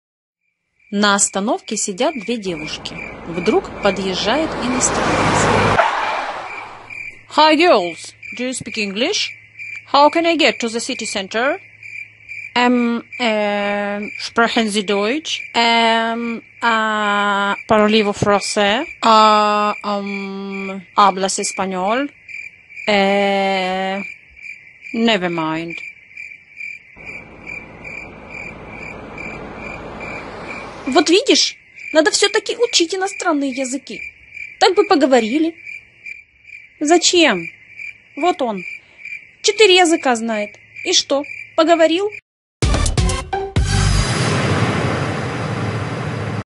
Прикольная получилась анимация в Pencil2D))) Две девушки и иностранец)))
О важности иностранных языков